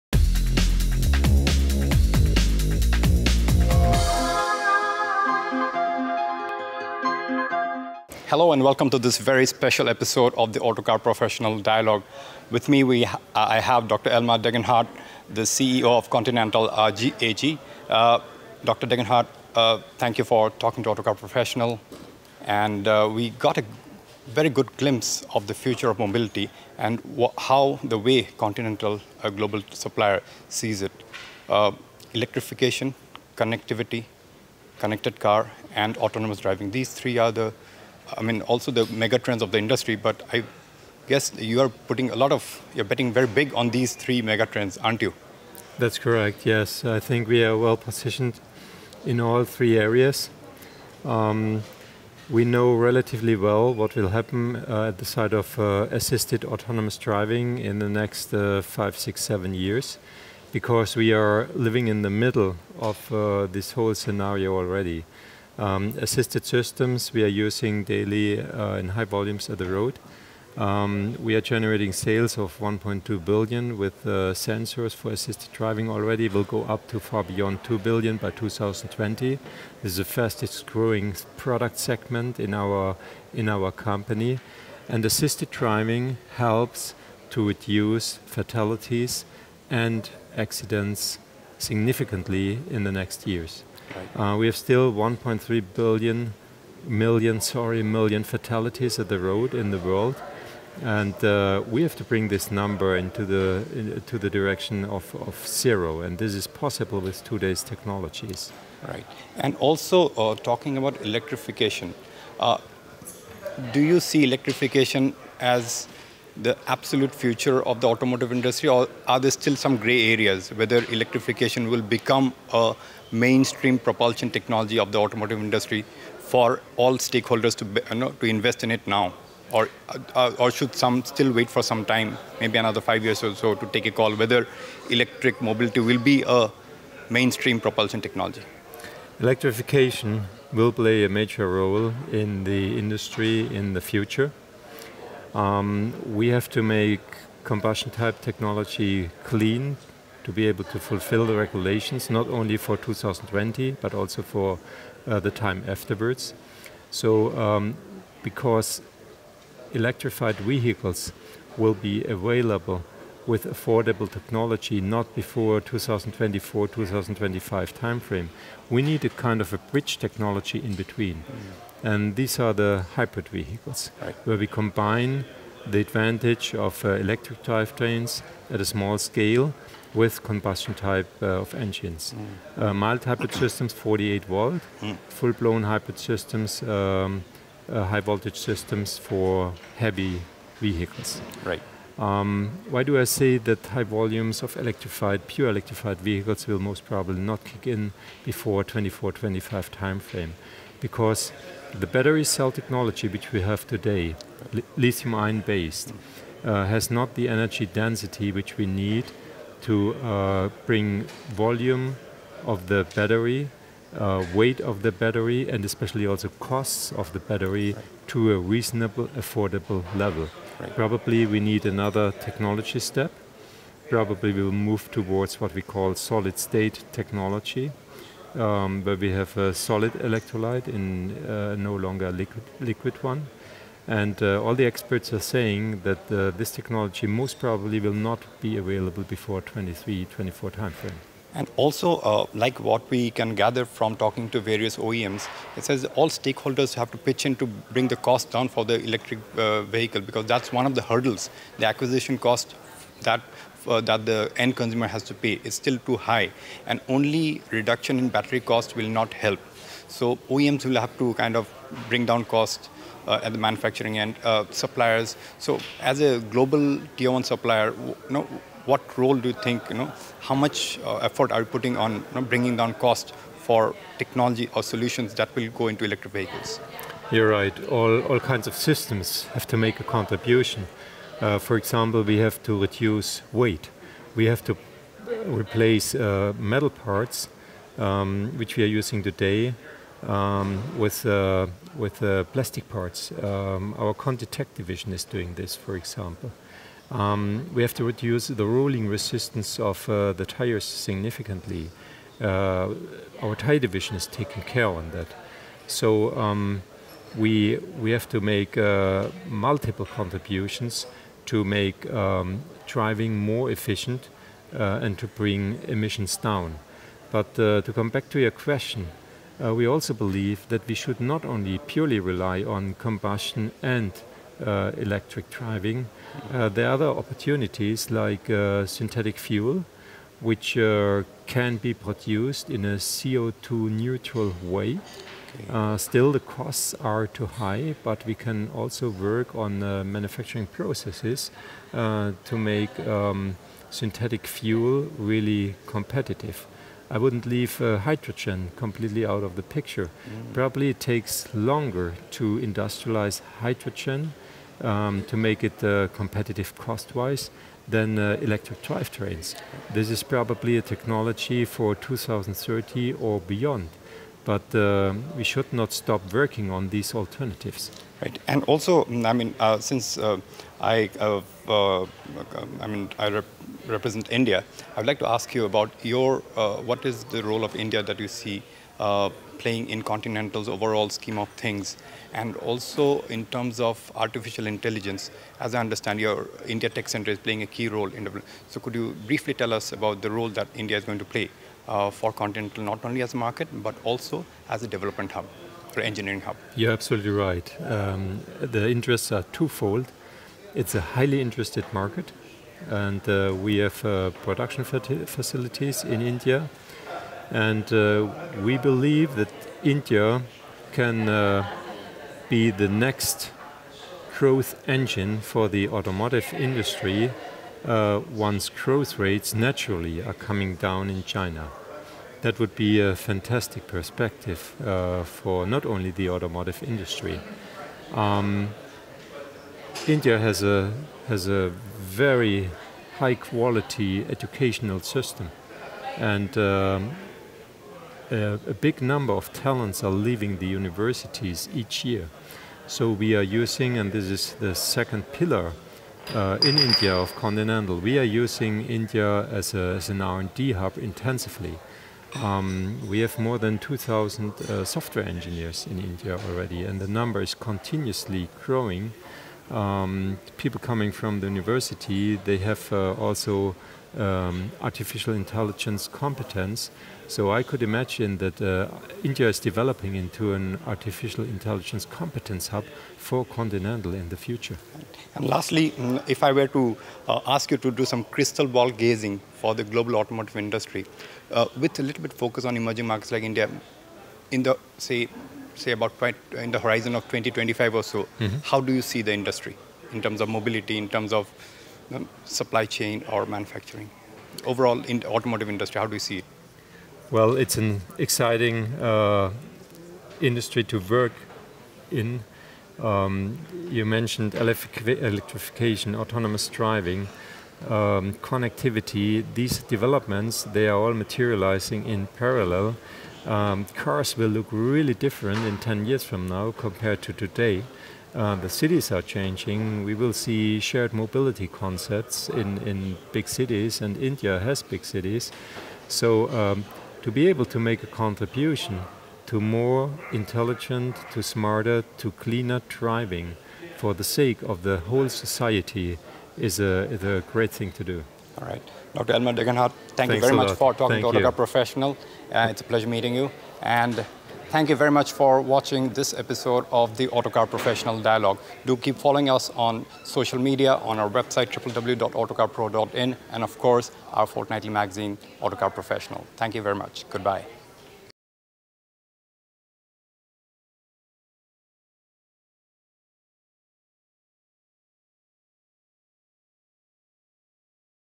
Dr Elmar Degenhart| Interview | Autocar Professional
Dr. Elmar Degenhart, Chairman of the Executive Board and CEO, Continental talks to Autocar Professional's Executive Editor Sumantra B Barooah about autonomous driving, electric mobility, why the EV industry will hit big volumes only after 2024, and the opportunities beyond electric mobility . In this special interview Dr. Degenhart talks about India possibly becoming the next growth engine for the automotive industry, and Continental's technical centre in Bengaluru becoming Continental's global Centre of Excellence for Artificial Intelligence.